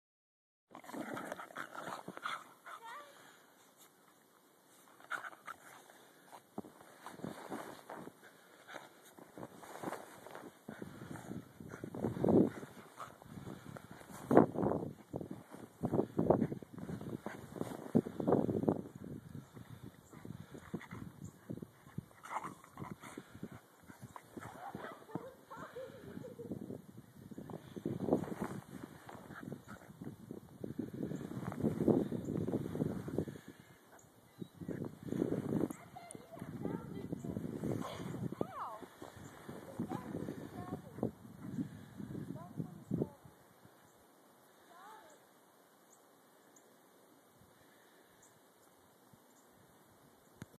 Jack Russells Playing In Snow December 2010 Christmas Snow
The day after Christmas and all were happy